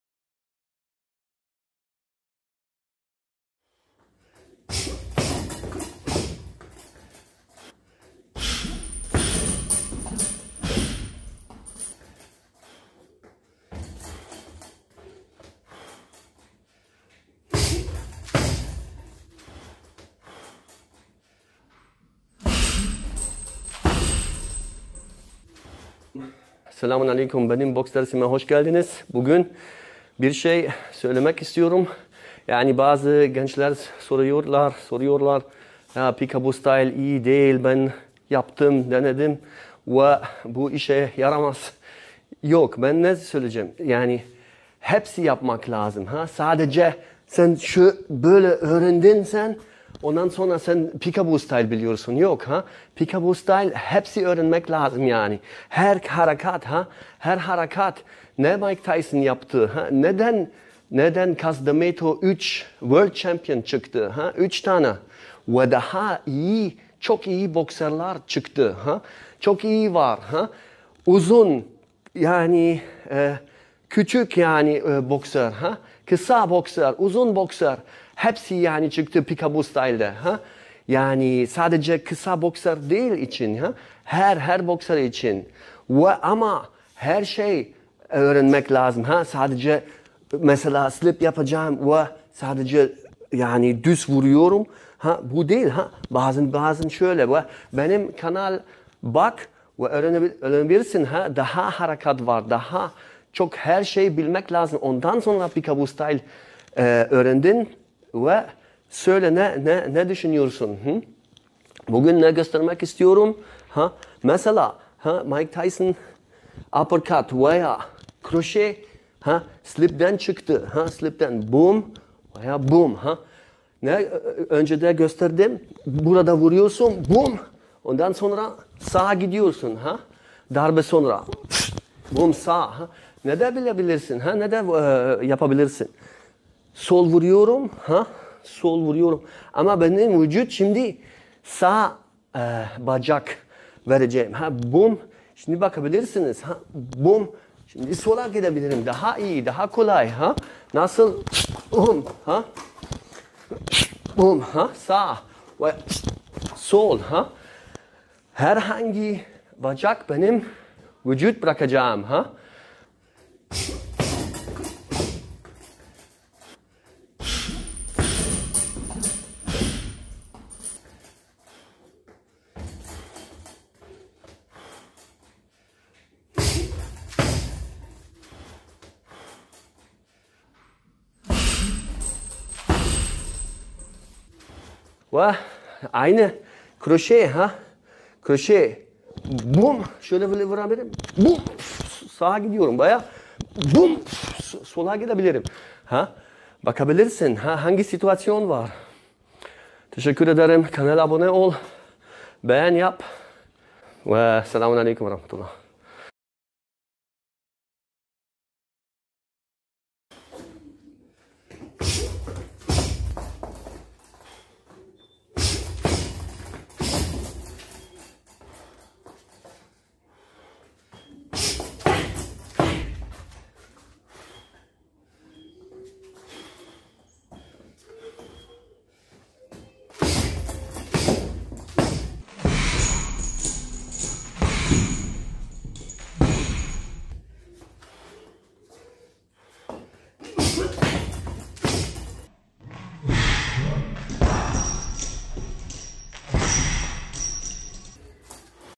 Peek A Boo stilini kimler uygulayabilir?
Polonyalı antrenör Thomas Tahir, Peek A boo stilinden bazı hareketleri gösteriyor ve Mike Tyson stilini kimlerin kullanabileceğini açıklıyor.